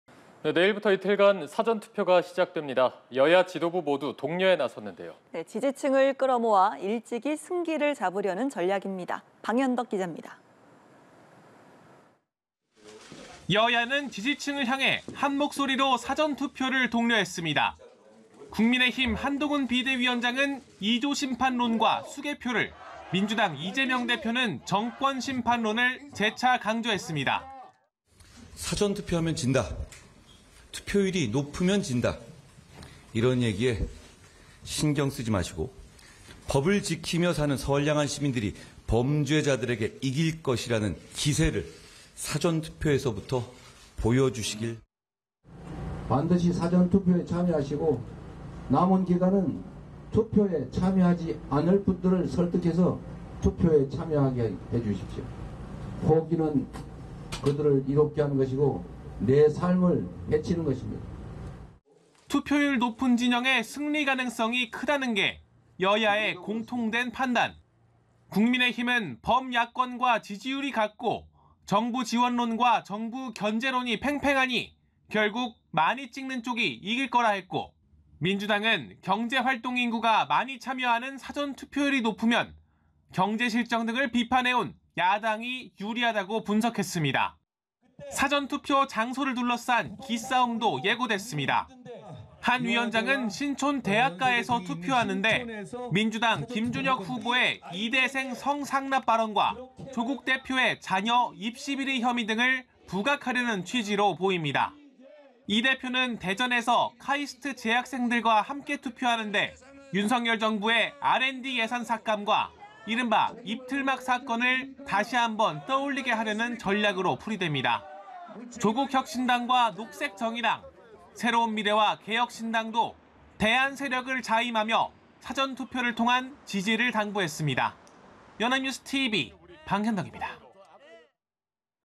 "사전투표하면 이긴다"…독려 나선 정치권 / 연합뉴스TV (YonhapnewsTV)
"사전투표하면 이긴다"…독려 나선 정치권
[뉴스리뷰]

[앵커]

내일(5일)부터 이틀간 사전투표가 시작됩니다.

여야 지도부 모두 독려에 나섰는데요.

지지층을 끌어모아 일찍이 승기를 잡으려는 전략입니다.

보도에 방현덕 기자입니다.

[기자]

여야는 지지층을 향해 한목소리로 사전투표를 독려했습니다.

국민의힘 한동훈 비대위원장은 '이조심판론'와 수개표를, 민주당 이재명 대표는 '정권심판론'을 재차 강조했습니다.

[한동훈 / 국민의힘 비상대책위원장] "사전투표하면 진다, 투표율이 높으면 진다 이런 얘기에 신경쓰지 마시고…법을 지키며 사는 선량한 시민들이 범죄자들에게 이길 것이라는 기세를 사전투표에서부터 보여주시기를…."

[이재명 / 더불어민주당 대표] "반드시 사전투표에 참여하시고 남은 기간은 투표하지 않을 분들을 설득해서 투표에 참여하게 해주십시오. 포기는 그들을 이롭게 하는 것이고 내 삶을 해치는 것입니다."

투표율이 높은 진영의 승리 가능성이 크다는 게 여야 공통 판단.

국민의힘은 범야권과 지지율이 같고 '정부지원론'과 '정부견제론'이 팽팽하니 결국 많이 찍는 쪽이 이길 것이라 했고, 민주당은 경제활동 인구가 많이 참여하는 사전투표율이 높으면 경제 실정 등을 비판해 온 야당에 유리하다고 분석했습니다.

사전투표 장소를 둘러싼 '기싸움'도 예고됐습니다.

한 위원장은 신촌 대학가에서 투표하는데, 민주당 김준혁 후보의 '이대생 성상납' 발언과 조국 대표의 '자녀 입시비리' 혐의 등을 부각하려는 취지로 보입니다.

이 대표는 대전에서 카이스트 재학생들과 함께 투표하는데, 윤석열 정부의 R&D 예산 삭감과 '입틀막' 사건을 다시 한 번 떠올리게 하려는 전략으로 풀이됩니다.

조국혁신당과 녹색정의당, 새로운미래와 개혁신당도 대안 세력으로 자리매김하게 해달라며 사전투표를 통한 지지를 당부했습니다.

연합뉴스TV 방현덕입니다.

[영상취재기자 신경섭·김성수·최성민·김상훈]